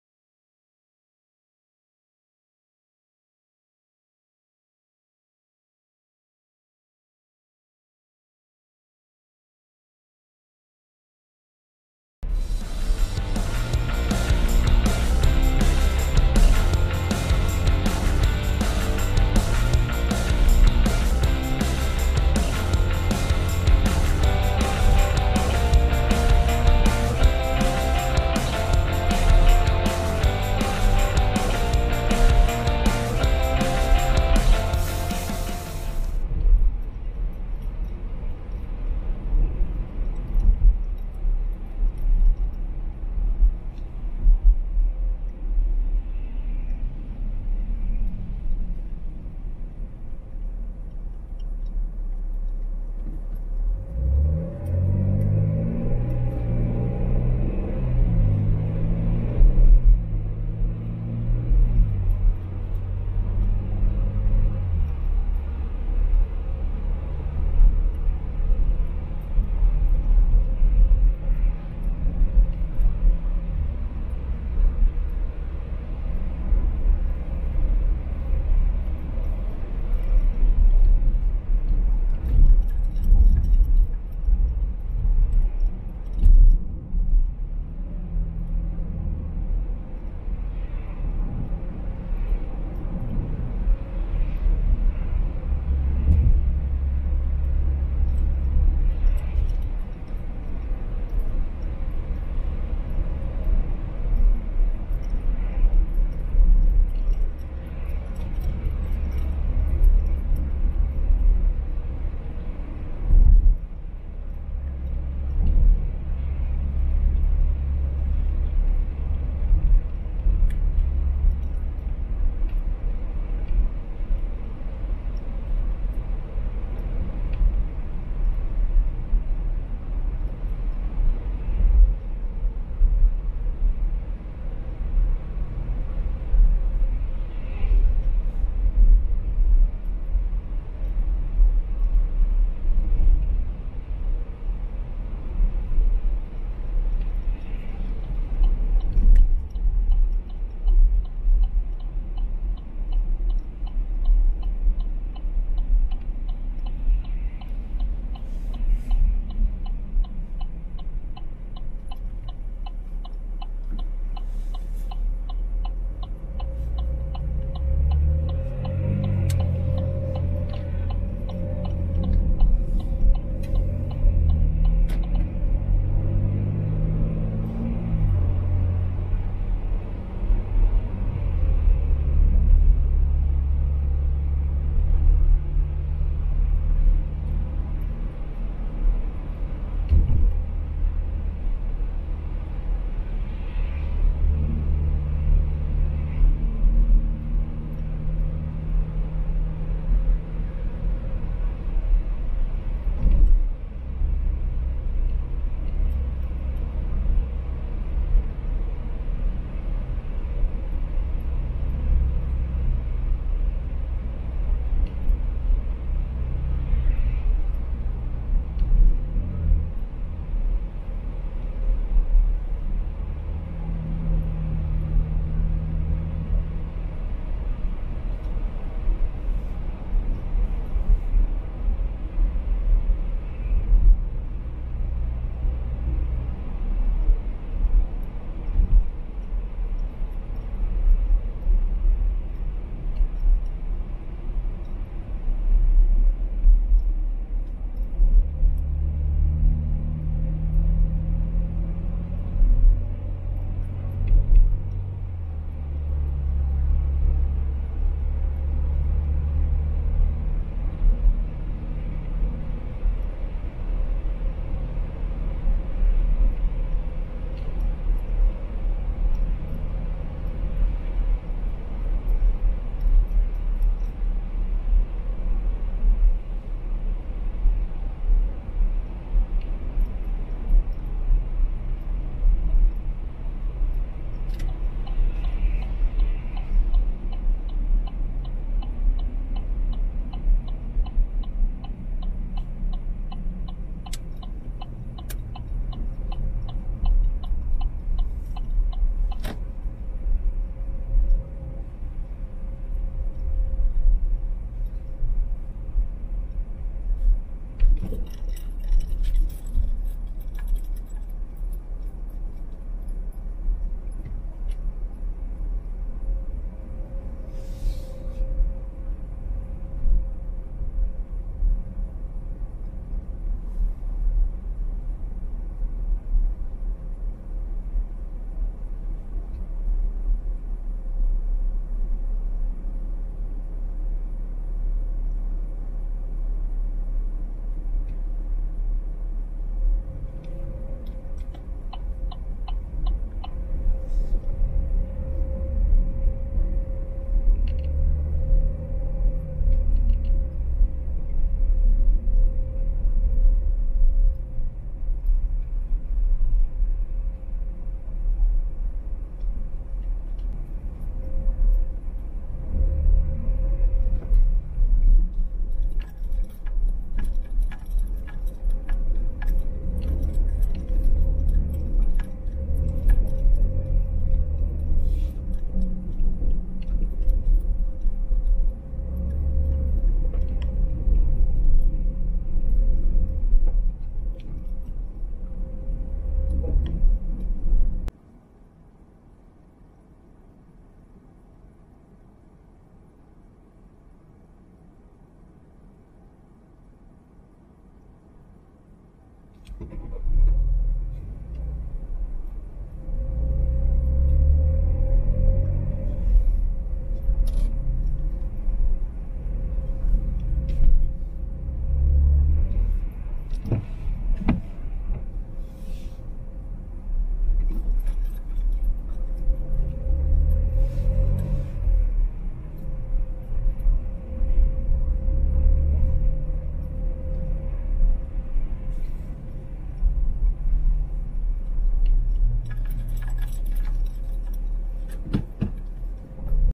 Ride To Spring Mill Manor Ivyland, Pa. From Rochboro, Pa. Armshouse Road.
Ride to Spring Mill Manor at Jacksonville Rd. Ivyland, Pennsylvania 18974. Party, Wedding, Gathering Events.